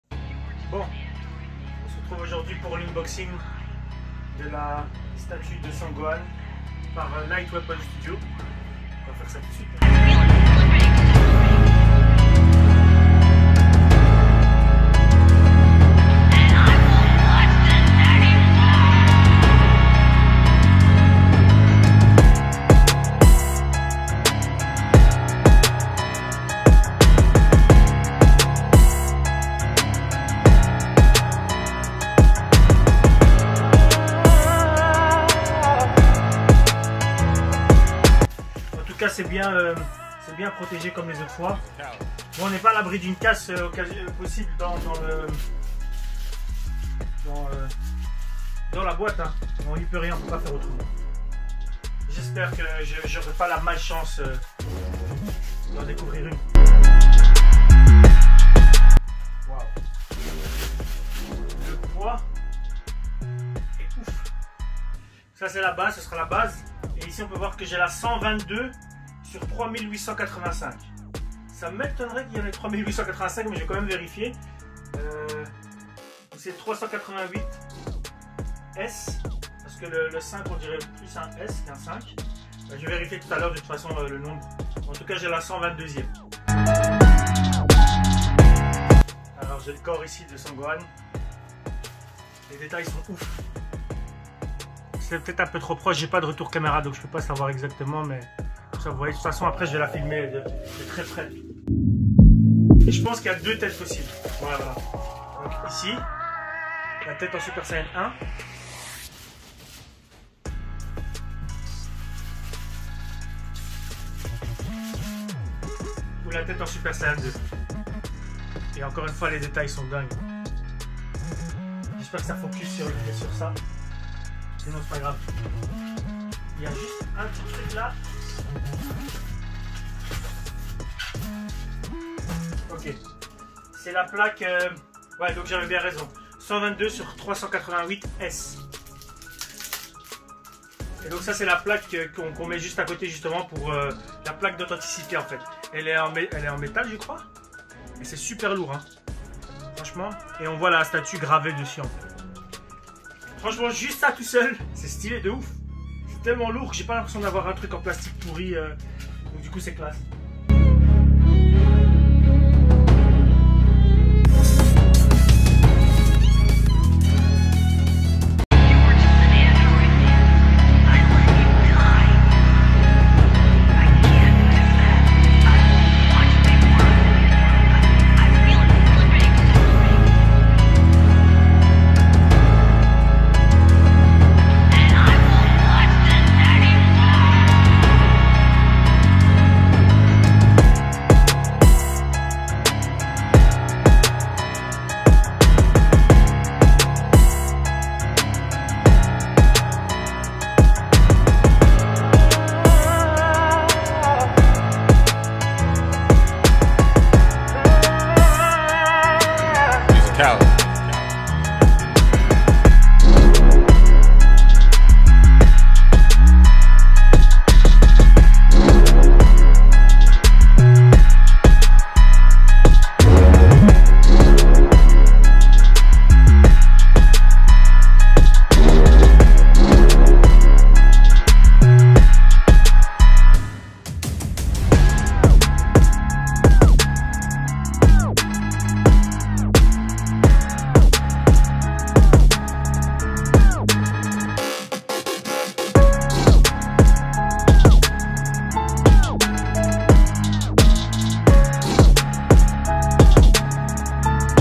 UNBOXING Son Gohan 1/6 | Light Weapon Studio
Voici mon Unboxing de la statue de SON GOHAN par LIGHT WEAPONS STUDIO !

N'hesitez pas à me suivre sur instagram et facebook, SUPERNESS ACTU